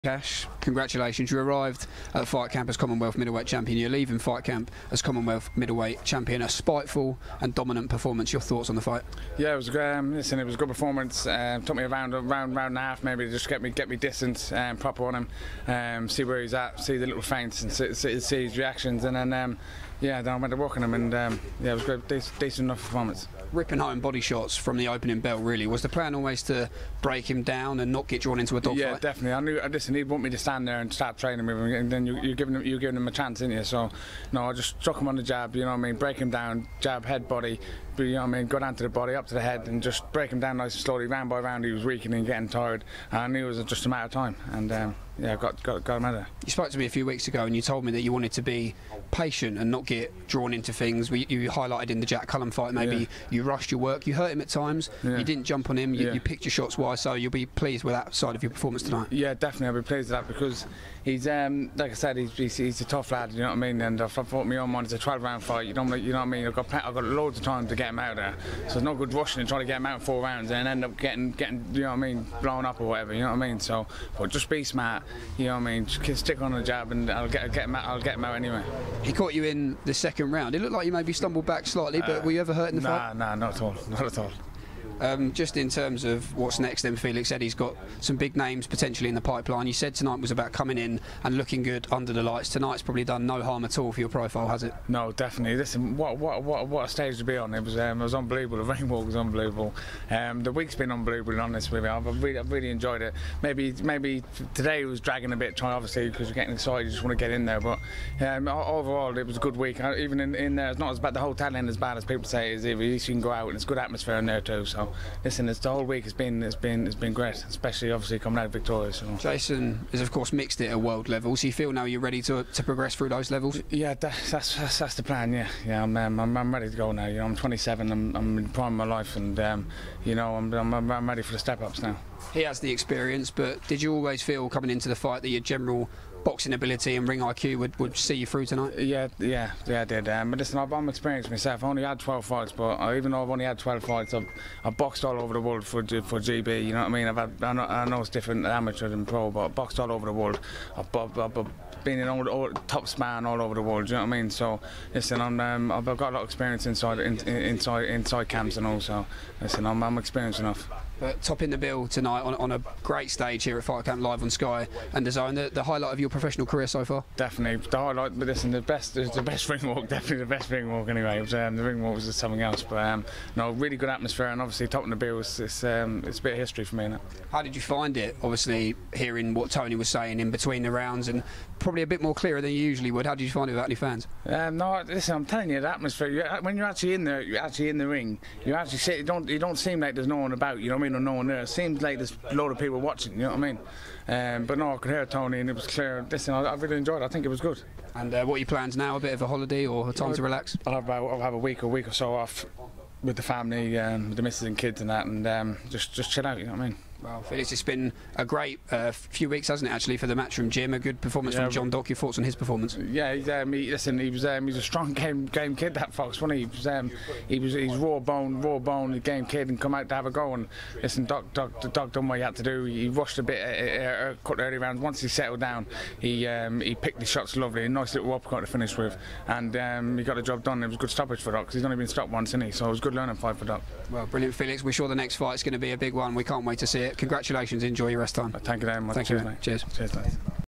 Interview: Spiteful Felix Cash destroys Jason Welborn at Fight Camp
Commonwealth Middleweight Champion Felix Cash puts on a show to retain his Title and push towards British and European Title contention. We catch up with Felix after the win to assess his performance...